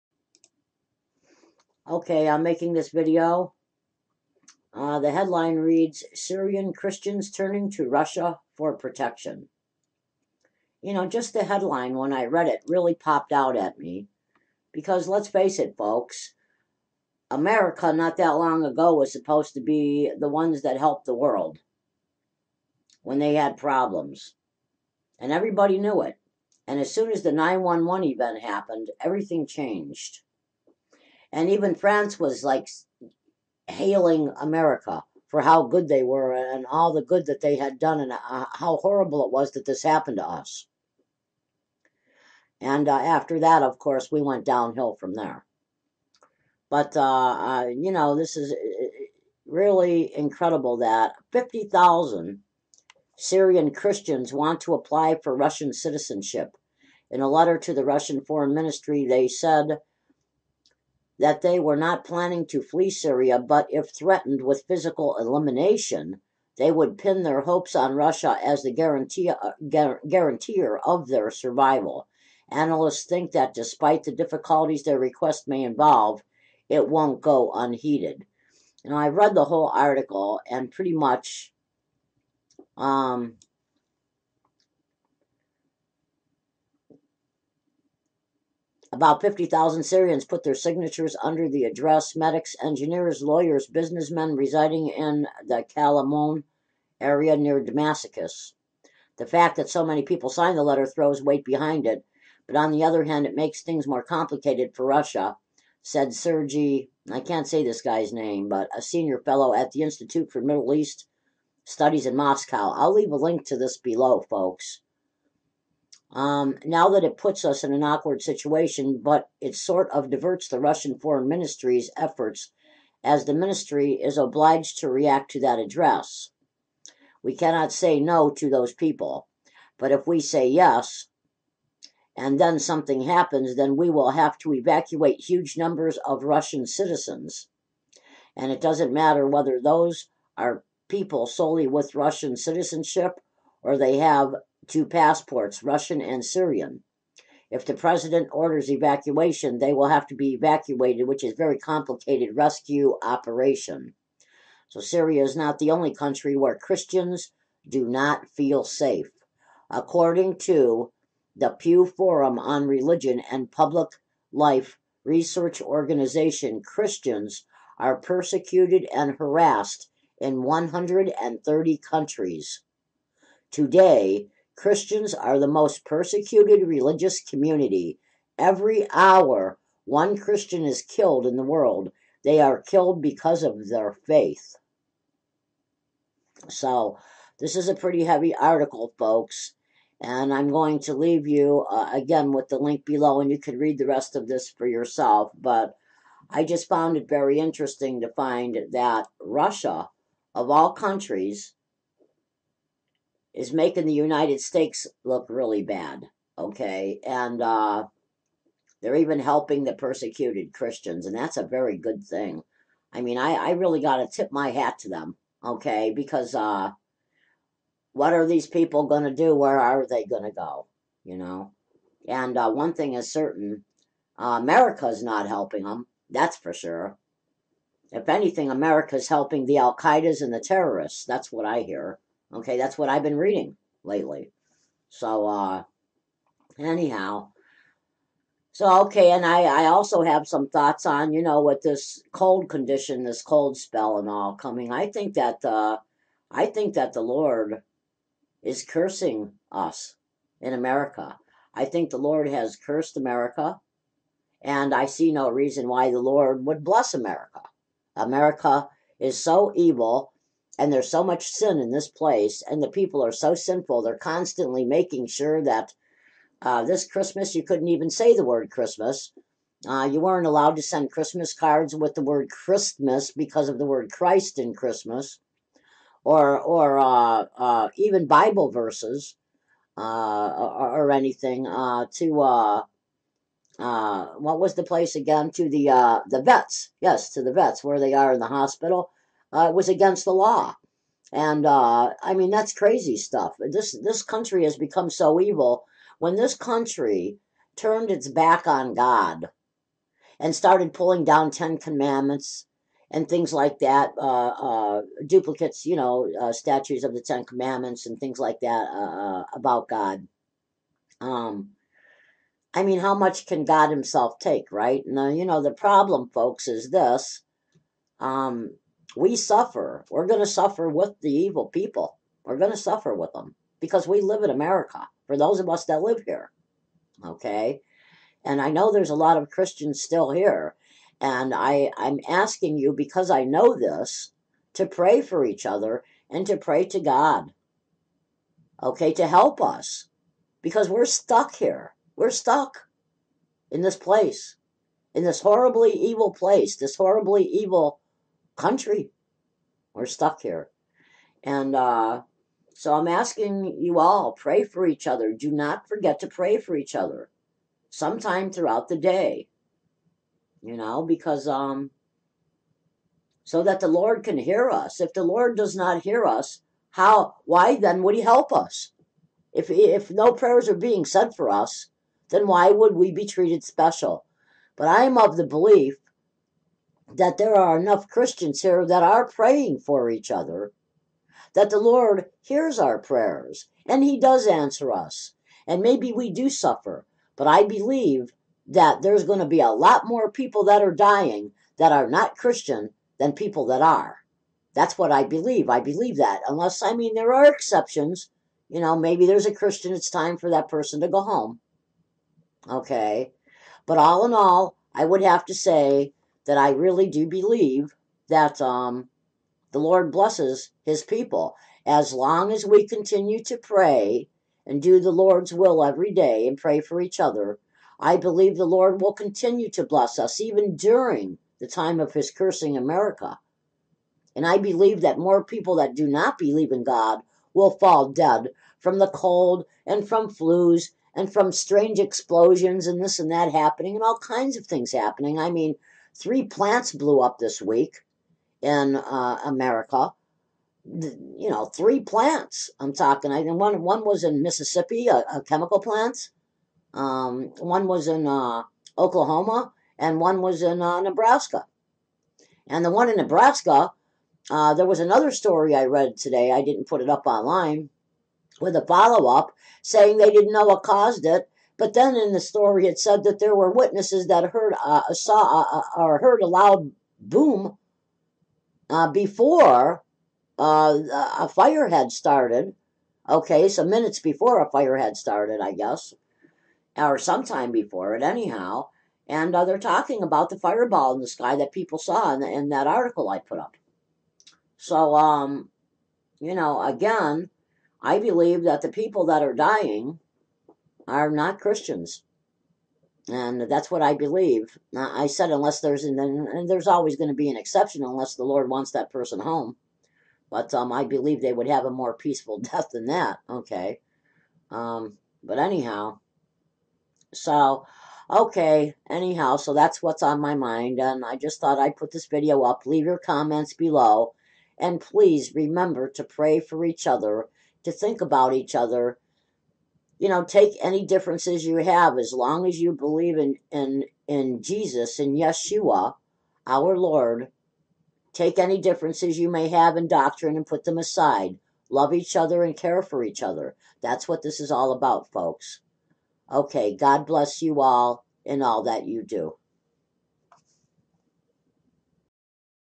Russia May Help 50,000 Persecuted Syrian Christians by AliyahEnoch
mirrored from: AliyahEnoch

Russia may be helping persecuted Syrian Christians. A time not so long ago, the United States used to help people, but no more.
Seems the only thing the politicians in America help, is how they can line their pockets with more of the people's money, to help themselves. There is no more America.
I must give credit where it is due. I thank God that someone will be helping these persecuted Christians...God bless them, because certainly America doesn't deserve a blessing from God.
Please listen to the video for more of my thoughts on this subject.
God bless you all and pray for each other, so our Lord Jesus, remembers us and keeps us safe, daily...